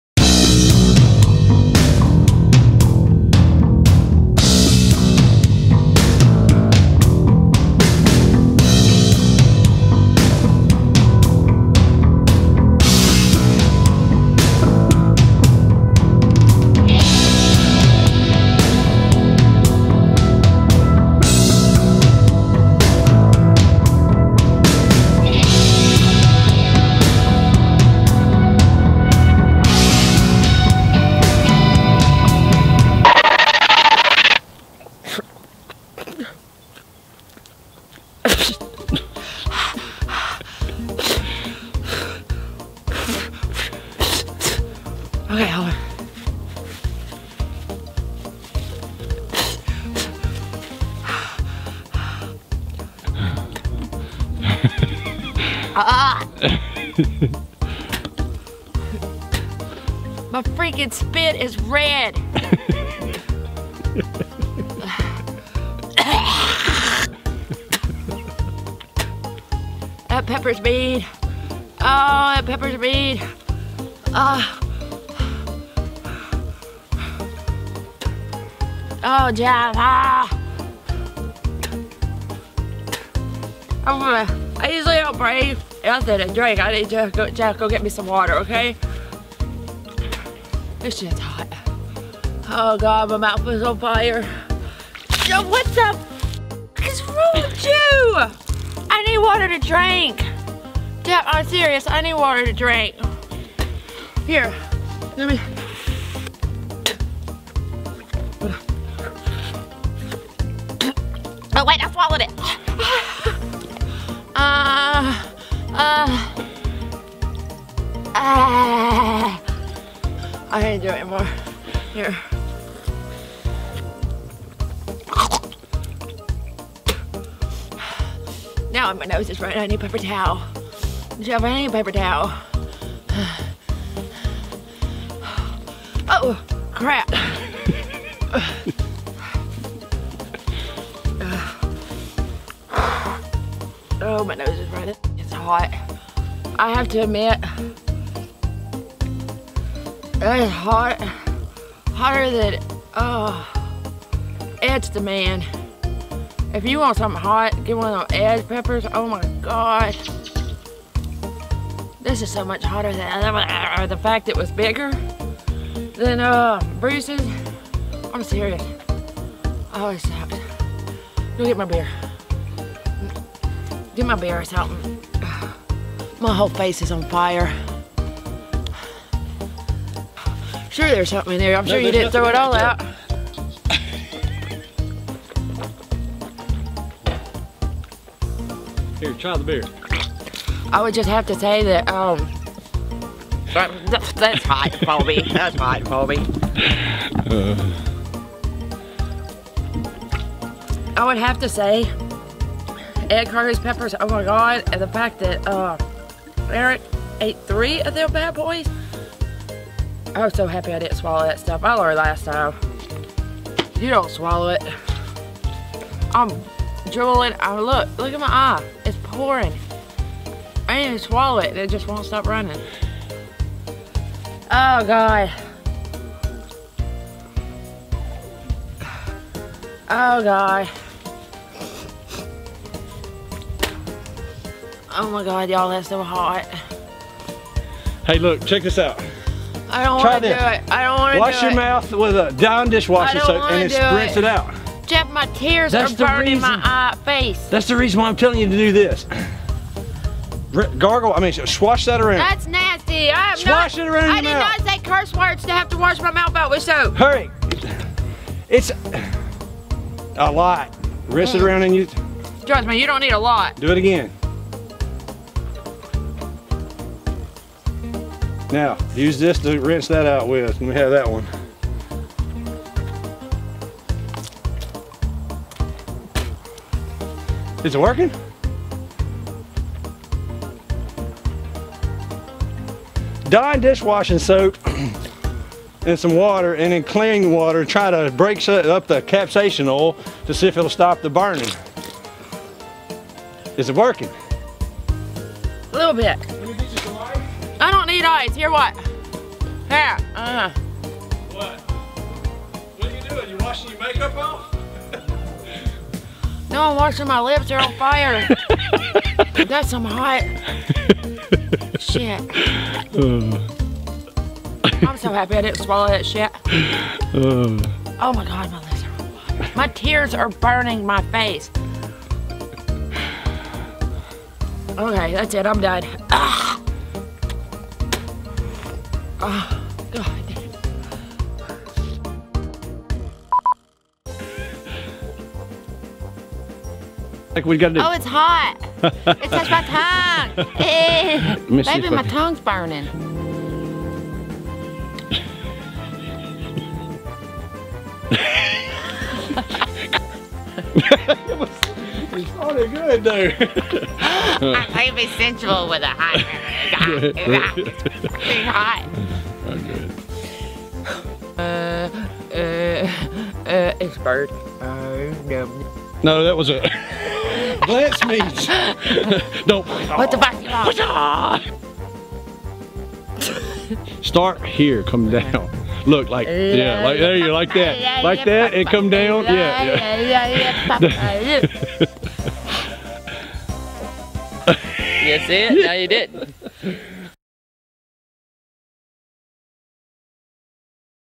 Girl Eat World Record Hottest Pepper (Ed Currie Reaper Pepper)
Heather finally meets her match! This pepper is way hotter than what she could handle!!!